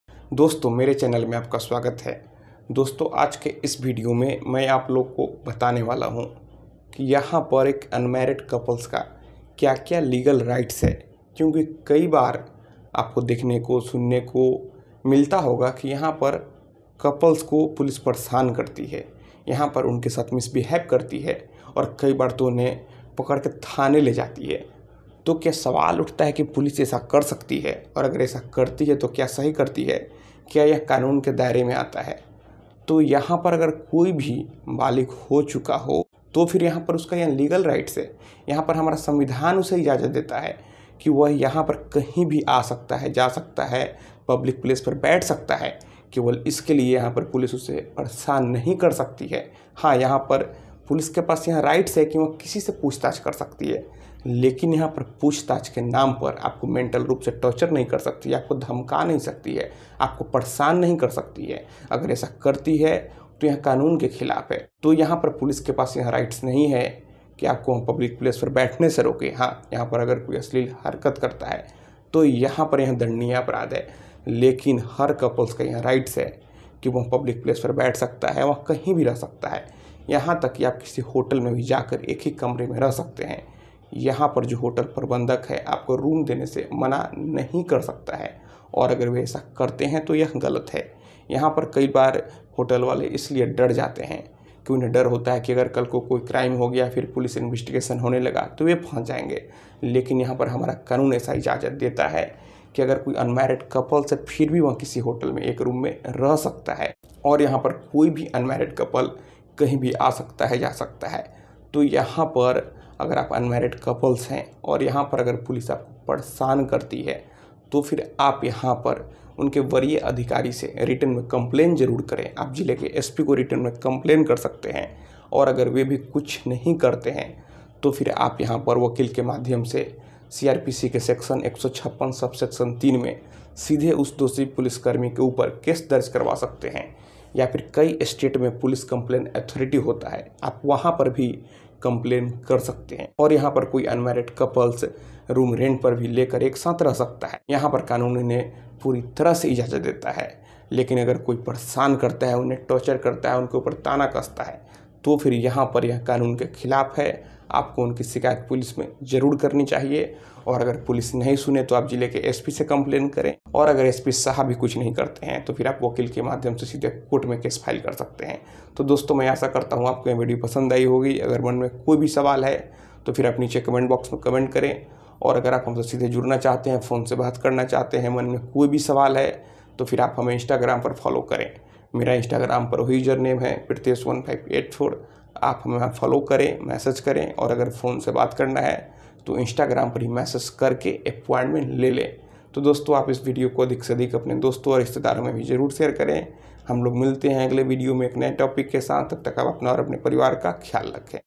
घूमते हुए कपल को पुलिस परेशान करे तो क्या करें।Ghumte hue couple ko police pareshan kare to kya kare
Ghumte hue couple ko police pareshan kare to kya kare